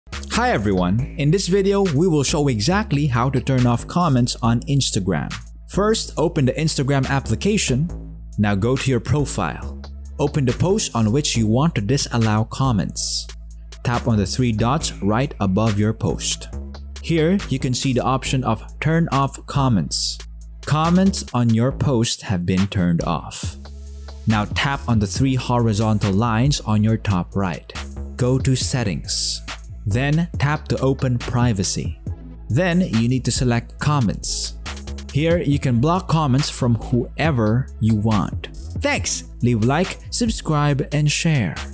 How To Turn Off Comments On Instagram IOS
In this video I will show you How To Turn Off Comments On Instagram IOS